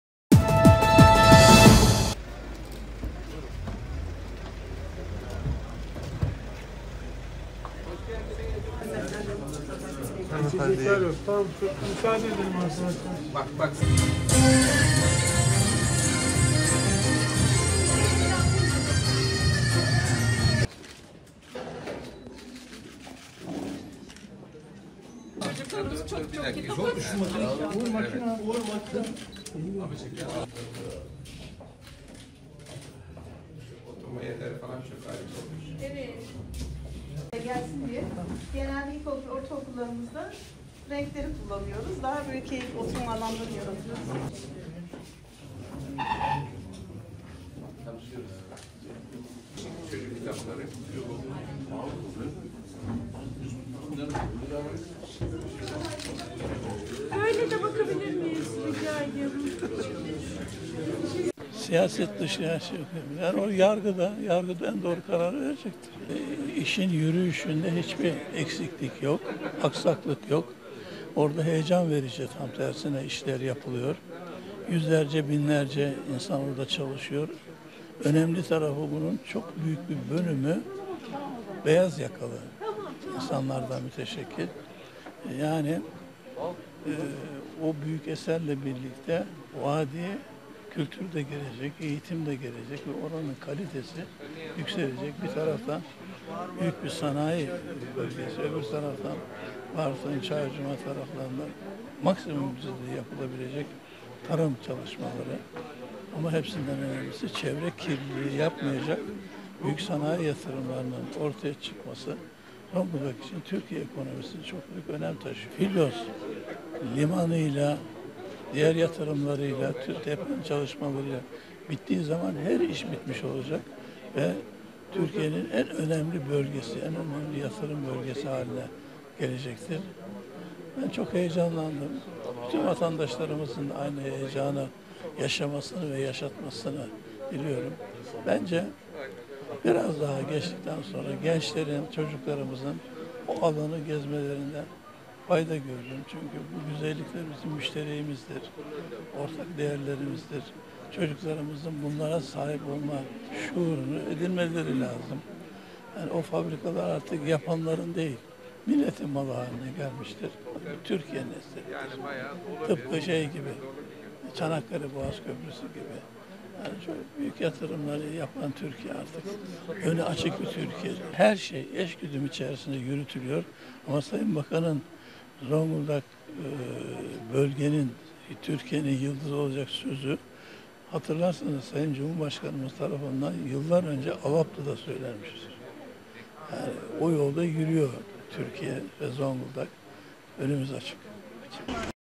Kütüphanesiz Okul Kalmasın Kapsamında, Köksal Toptan İlkokulu Ve Ortaokulu Kütüphanesinin Açılışı
Cumhurbaşkanlığı Yüksek İstişare Kurulu (YİK) Üyesi Köksal Toptan, Filyos bölgesindeki çalışmalara ilişkin, "Orada heyecan verici işler yapılıyor. Yüzlerce, binlerce insan orada çalışıyor." dedi.
Toptan, Cumhurbaşkanı Recep Tayyip Erdoğan'ın eşi Emine Erdoğan'ın himayesinde başlatılan "Kütüphanesiz Okul Kalmasın Projesi" kapsamında, İnağzı Mahallesi'ndeki Köksal Toptan İlkokulu ve Ortaokulu'nun kütüphanesinin açılışına katıldı.
Hayırsever avukat Ali Osman Odabaş'ın, eşi avukat Hikmet Odabaş'ın anısına oluşturduğu kütüphanenin açılışı öncesinde öğrenciler halk oyunu gösterisinde bulundu.
Açılış kurdelesinin kesilmesinin ardından Toptan ve beraberindekiler kütüphaneyi gezip, yetkililerden bilgi aldı.
Köksal Toptan, kütüphanenin öğrencilere hayırlı olmasını diledi.
Kentte daha güzel şeyler yapma imkanı bulacaklarını belirten Toptan, "Eğitim alanında mükemmel bir noktadayız. Üniversitemiz, Sayın Valimiz ve Belediye Başkanımız büyük bir gayret içerisinde. Bu takım bize lig atlatacaktır. Dün Filyos bölgesini dolaştım. Orada gördüklerim Zonguldak'ın da lig atlayacağını bize göstermektedir. Orada da harika işler yapılıyor. Türkiye ekonomisinin ve geleceğinin daha parlak, daha büyük, daha başarılı olması için şu anda orada 4 bin kişi çalışıyor. Yakında belki de daha da çok insan çalışacak." ifadesini kullandı.